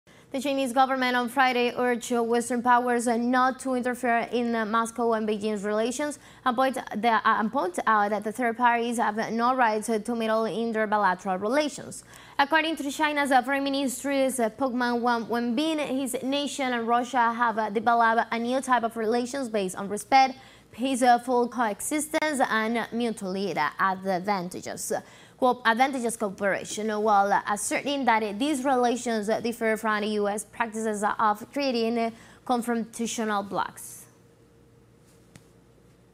Chinese government calls for respect for bilateral relations between Moscow and Beijing
The Chinese government on Friday urged Western powers not to interfere in relations between Moscow and Beijing, saying that third parties have no right to interfere in their bilateral relations. teleSUR